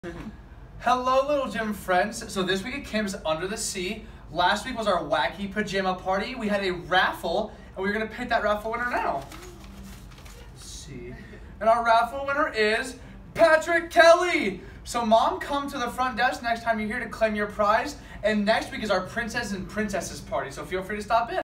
Wacky Week Pajama Party Raffle Winner!
Thanks to all of the kids and parents that participated in our Pajama Party by wearing their PJ's to camps and classes last week! Congrats to the Kelly family! Claim your prize the next time you're in the gym!